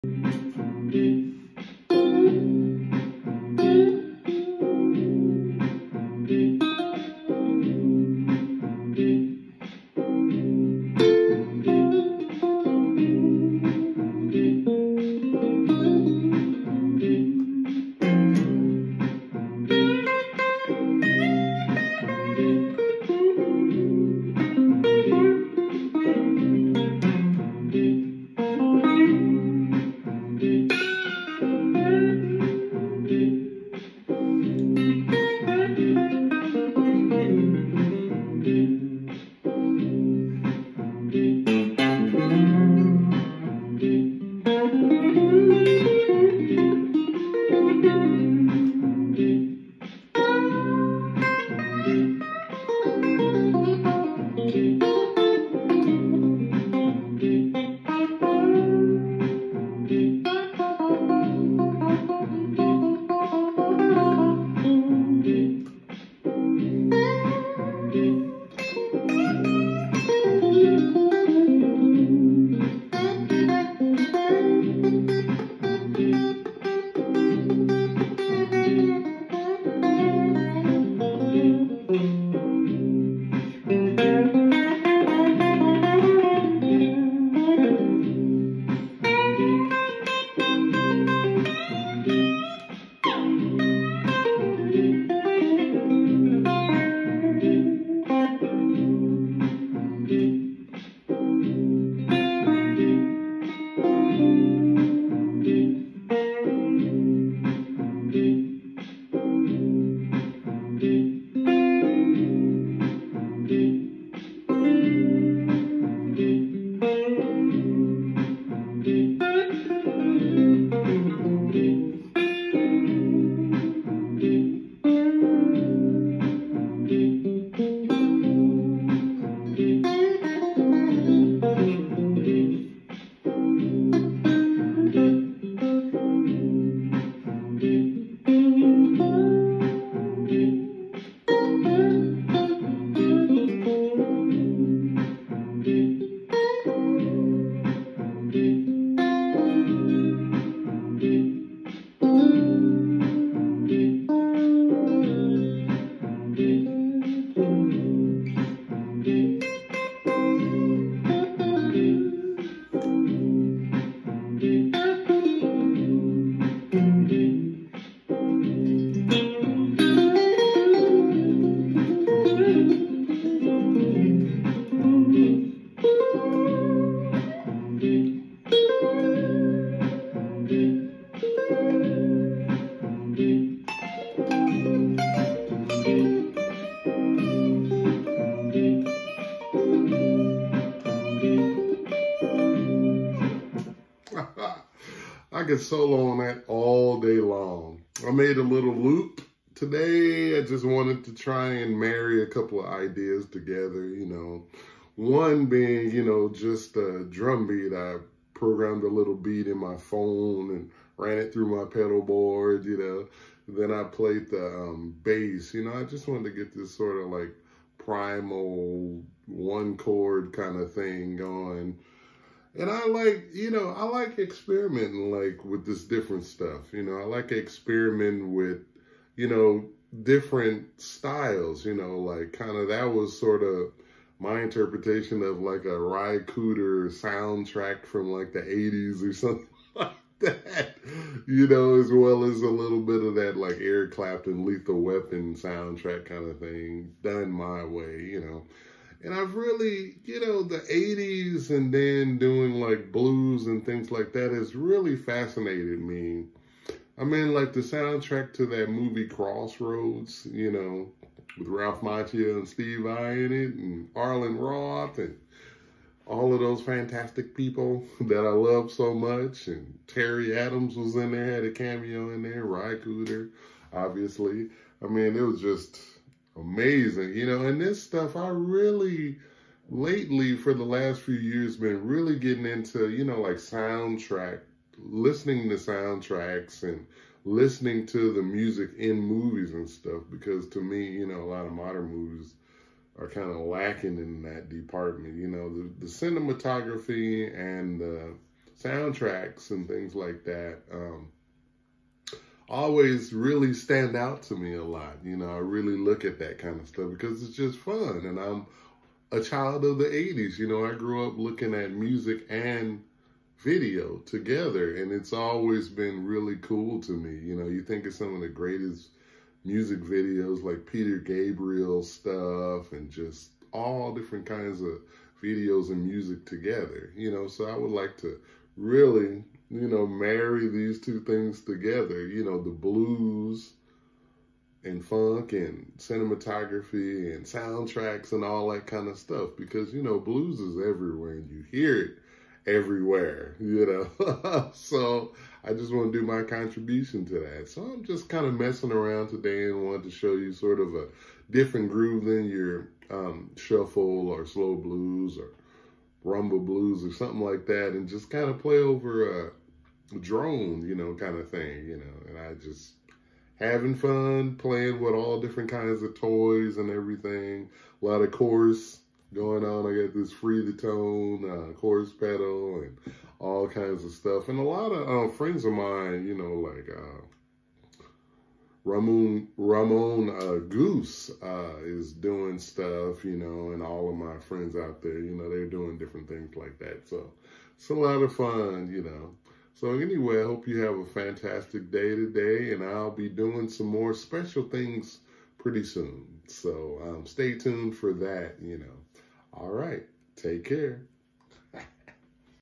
MOVIE SOUNDTRACK, MUSIC VIDEOS AND THE BLUES "THINKING OUTSIDE THE BOX" WITH KIRK FLETCHER
Blues musician Kirk Fletcher plays over a one chord vamp he made with a loop pedal. His inspiration came from 80's soundtracks that marry things like modern sounds of the time with more rural textures. Movies like Crossroads, Paris Texas, Lethal Weapon etc. incorporate some of these ideas to name a few. These are the sounds of Kirk's youth. Enjoy! @PRS Guitars